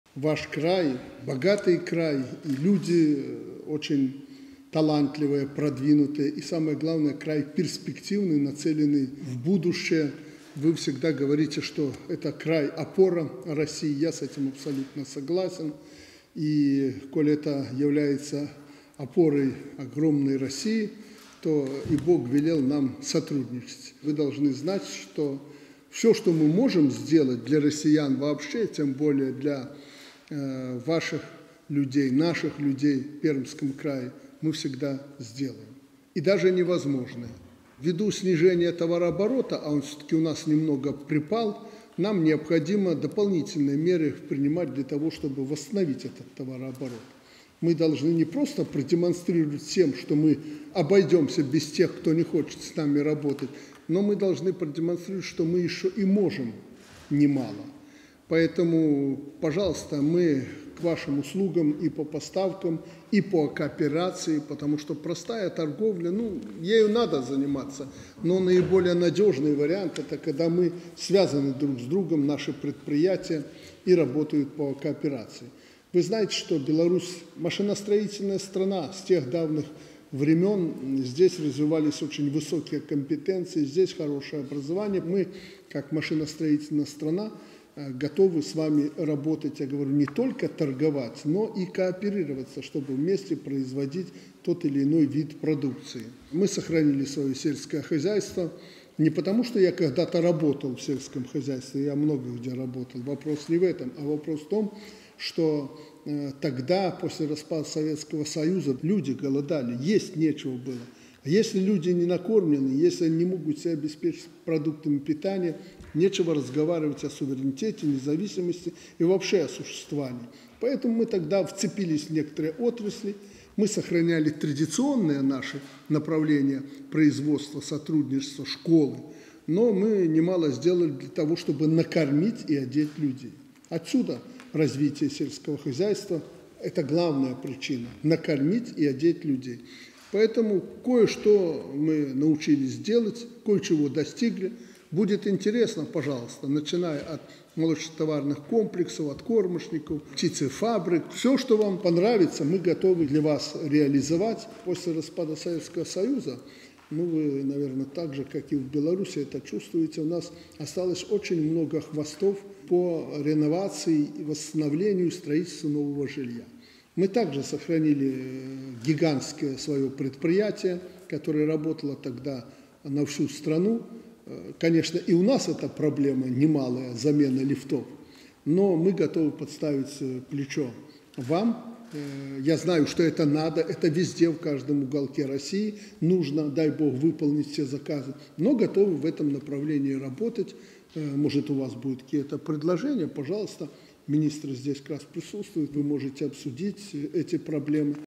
Лукашенко: И у нас эта проблема немалая! Но мы готовы подставить плечо вам!
Лукашенко: Вы должны знать, что всё, что мы можем сделать для россиян вообще, и тем более для ваших людей, наших людей в Пермском крае, мы всегда сделаем! И даже невозможное!

Беларусь готова к активному развитию сотрудничества с Пермским краем России. Об этом глава государства Александр Лукашенко заявил 28 июля на встрече с губернатором Пермского края России Дмитрием Махониным.